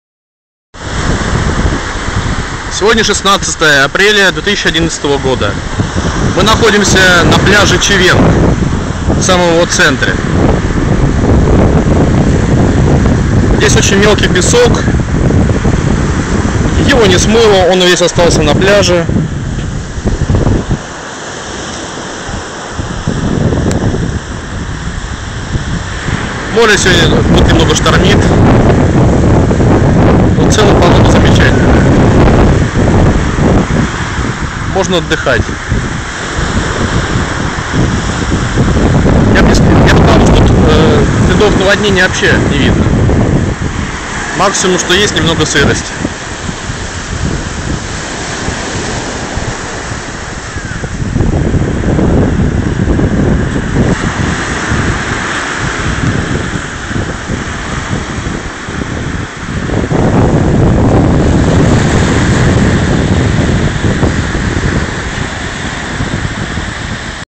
Beach Chaweng in Koh Samui 16 april 2011 after flood
Пляж в центре Чавенга 16 апреля 2011 года после наводнения. Beach Chaweng in Koh Samui 16 april 2011 after flood. Все фото и ведео о Самуи на